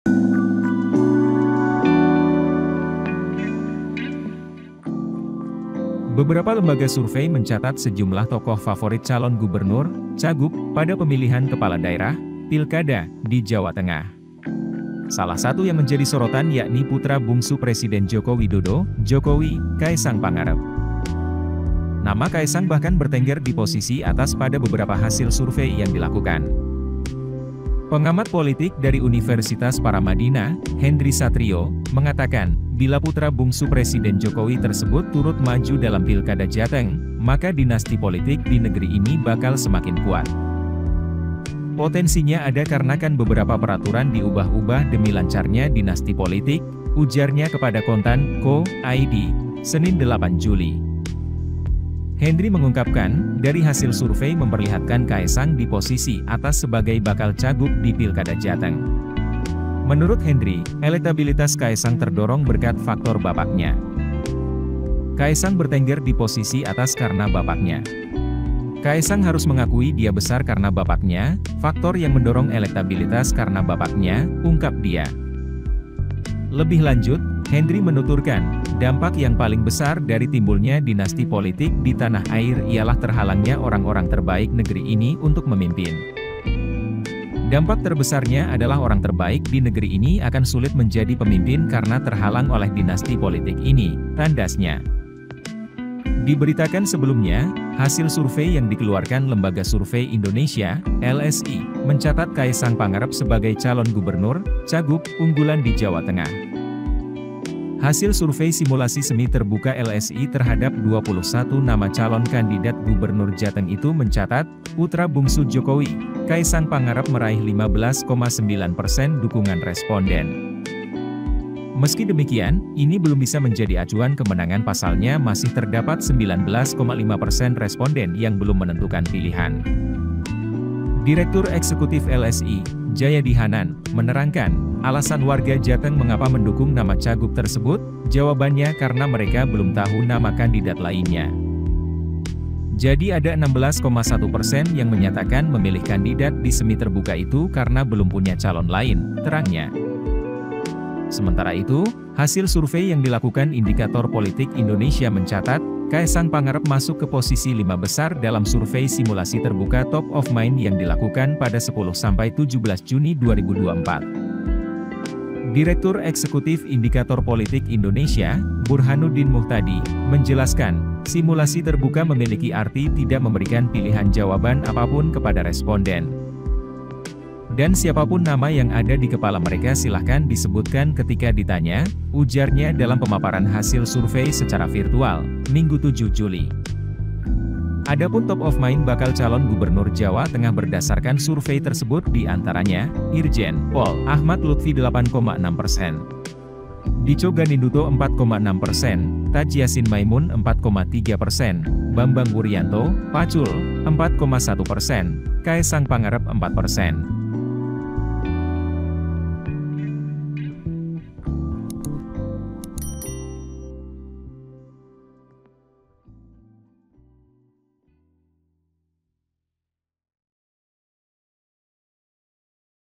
Kaesang di Posisi Atas Cagub Favorit di Jateng, Dinasti Politik Berlanjut? | Kontan News
Beberapa lembaga survei mencatat sejumlah tokoh favorit calon gubernur (cagub) pada pemilihan kepala daerah (Pilkada) di Jawa Tengah. Salah satu yang menjadi sorotan yakni putra bungsu Presiden Joko Widodo (Jokowi), Kaesang Pangarep.

Nama Kaesang bahkan bertengger di posisi atas pada beberapa hasil survei yang dilakukan. Pengamat Politik dari Universitas Paramadina, Hendri Satrio, mengatakan, bila putra bungsu presiden Jokowi tersebut turut maju dalam Pilkada Jateng, maka dinasti politik di negeri ini bakal semakin kuat.

“Potensinya ada karena kan beberapa peraturan diubah-ubah demi lancarnya dinasti politik,” ujarnya kepada Kontan.co.id, Senin (8/7).